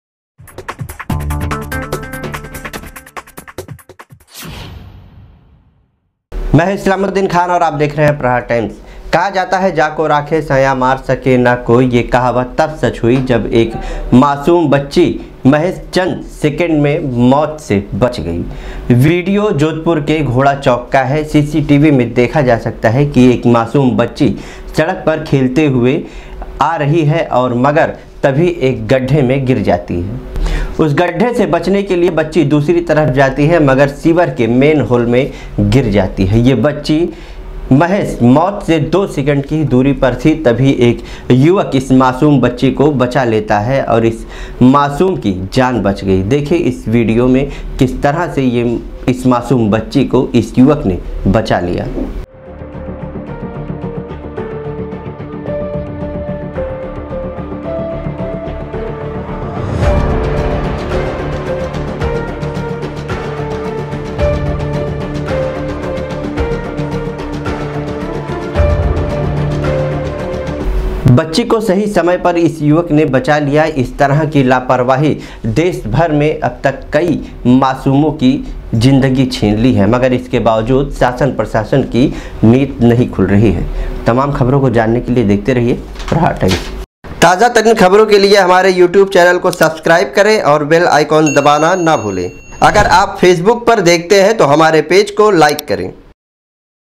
दो सेकेंड मौत से दूर,युवक ने कैसे बचाई जान | islamuddin khan | Aapka Prahar Times -
दो सेकेंड मौत से दूर,युवक ने कैसे बचाई जान | islamuddin khan,
खुले सीवरेज में गिरी मासूम बच्ची

मौके पर युवक ने बचाई बच्ची की जान,